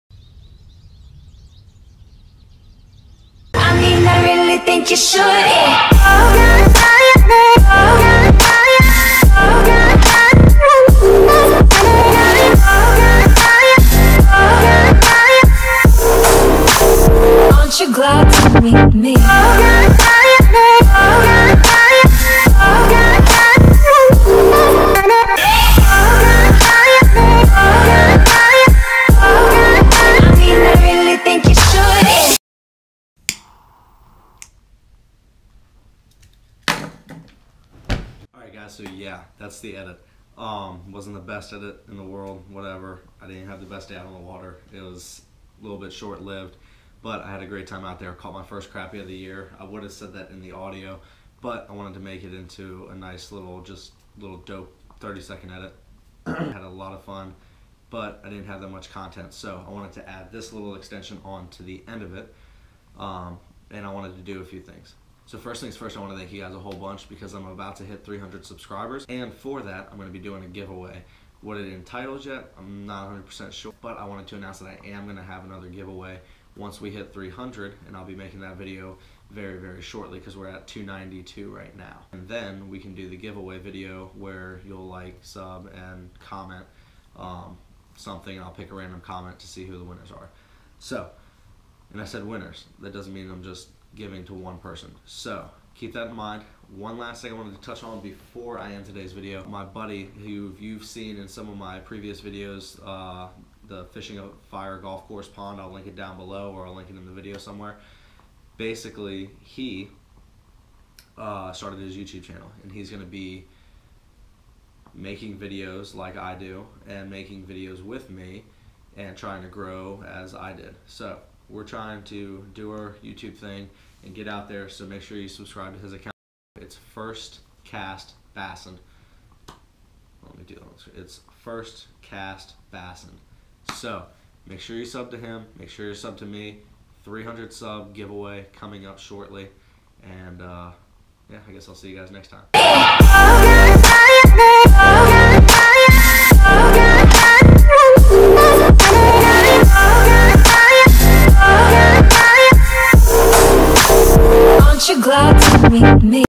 Catching the Smallest Bass in the World + 300 SUB GIVEAWAY
Well.. this video didn't really go as planned but it turned out to be a nice little mini edit. Hope you all enjoyed and checkout the next one!  WE ARE ALMOST AT 300 SUBSCIBERS! CRUSH THAT SUB AND LIKE BUTTON AND HELP THIS CHANNEL GROW!

My Gear: